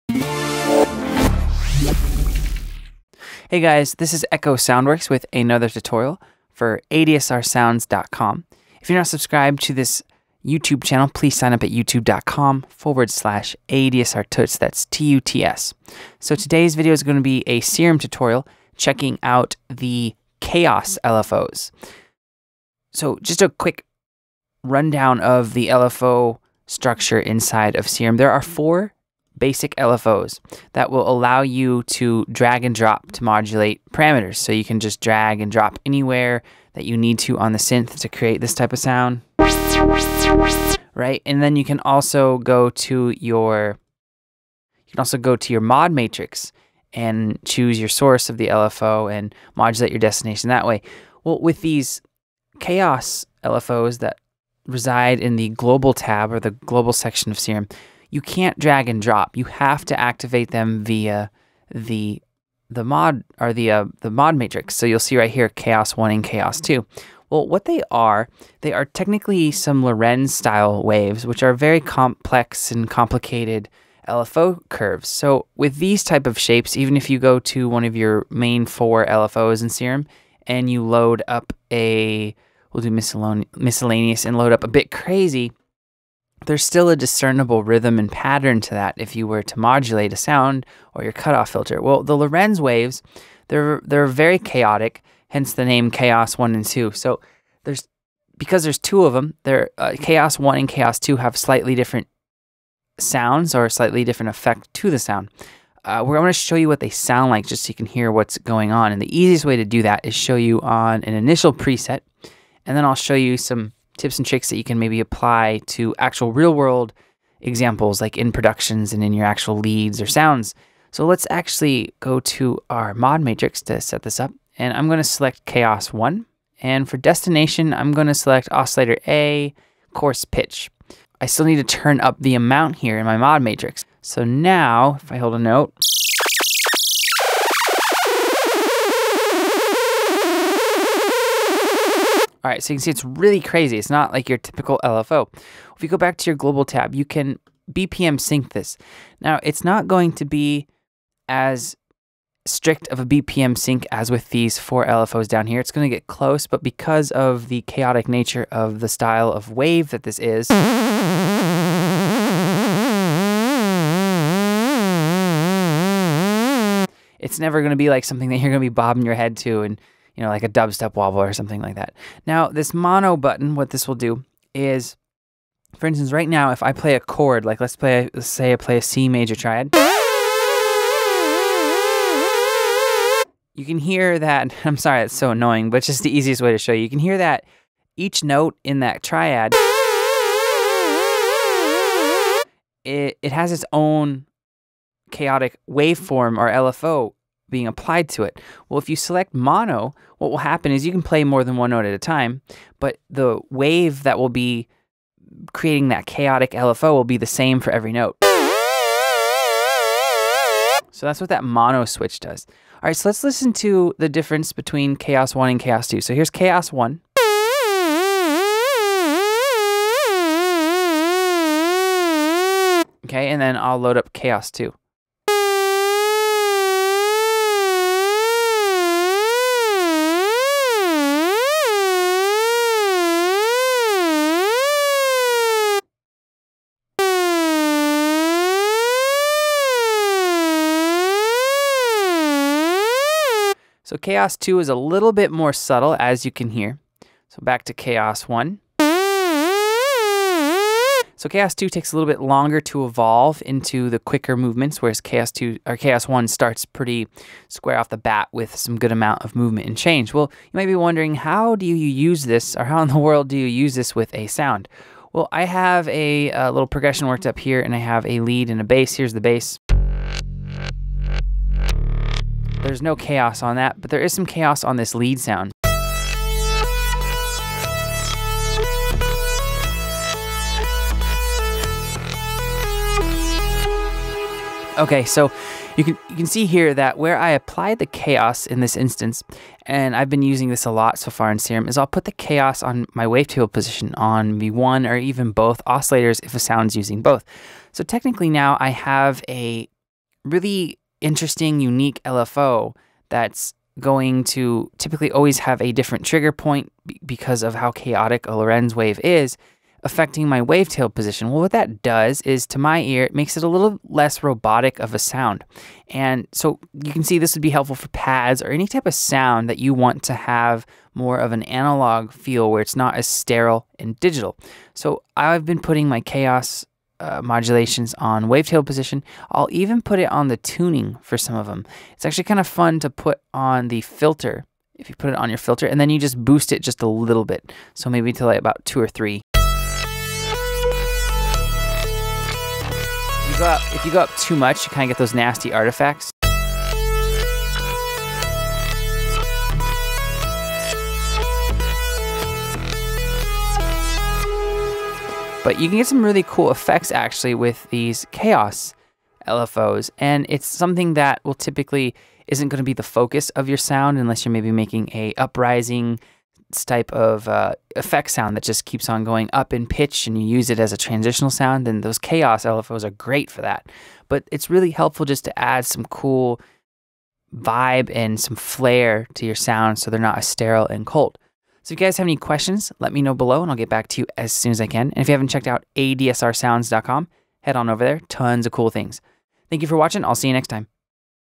Serum Tutorial - Chaos LFO's
In this Xfer Records Serum tutorial checking out the chaos LFO's

Echo Sound Works walks through the Chaos LFO's in Serum. These are definitely an interesting tool to use!